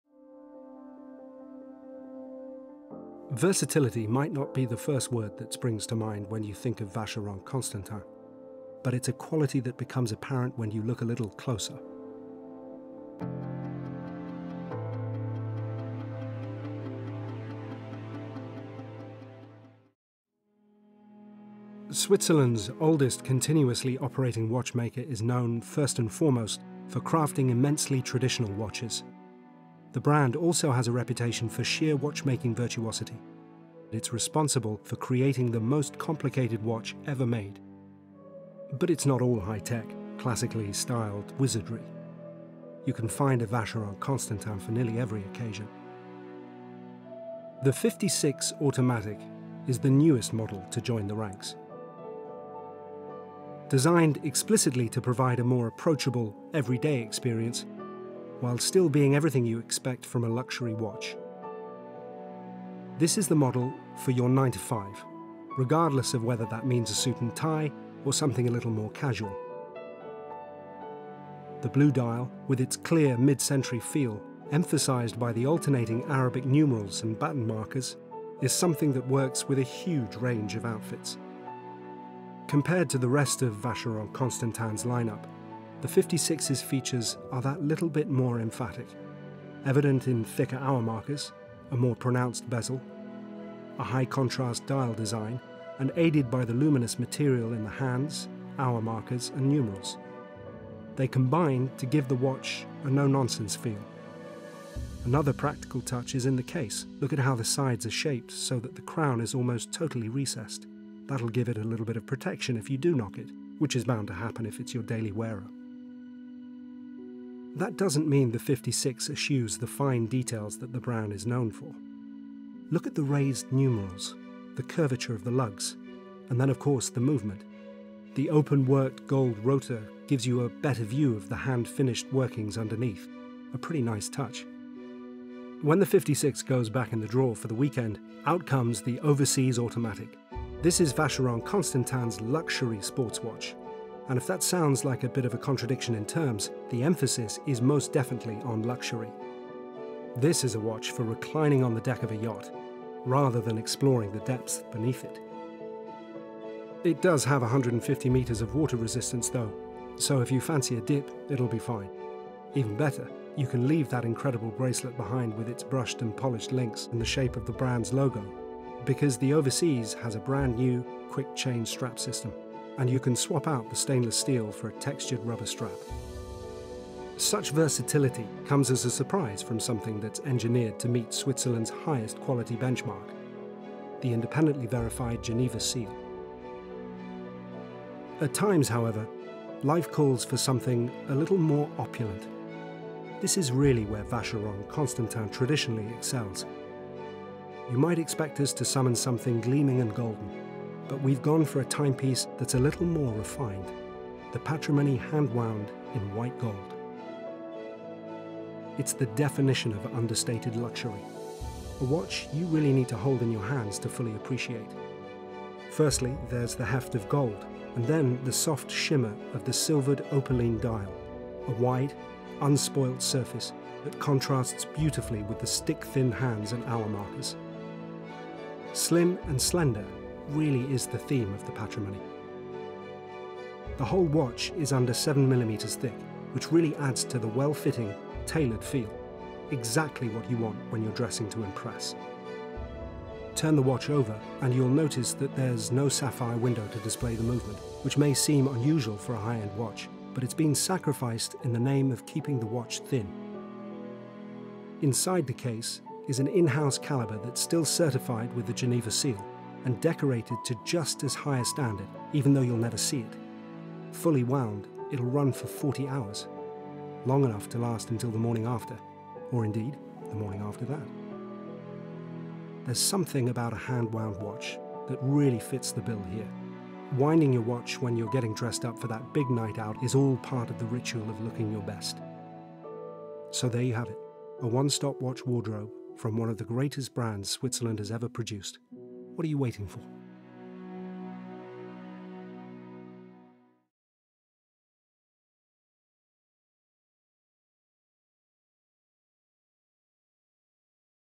Work, Rest And Play with Vacheron Constantin | MR PORTER
Could you choose a three-watch wardrobe to cover as many bases as possible, restricting your choices only to one brand? That was the task we set ourselves when we filmed our ‘Work, Rest, and Play’ series with Vacheron Constantin – a FiftySix Automatic for the everyday, the Overseas Chronograph for our downtime, and the Patrimony Hand-Wound for special occasions. But everyone has their own answer: what’s yours?